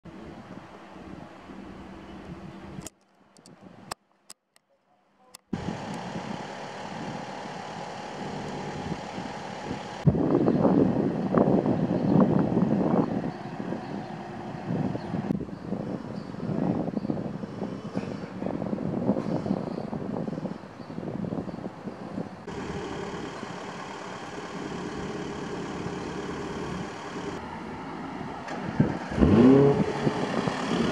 Auto's botsen frontaal Nieuw Amsterdam
NIEUW-AMSTERDAM ~ Aan de Zijtak OZ heeft zich zaterdagmiddag omstreeks 15.35 een ongeval plaatsgevonden.

Bij het ongeval waren meerdere voertuigen betrokken,  deze raakten zwaar beschadigt.

De politie en ambulance kwamen met spoed ter plaatse.

De betrokkenen worden door het medisch personeel van de ambulance onderzocht.

De politie zorgt voor een veilige omgeving.

Wat de oorzaak van het ongeval is, is niet bekend.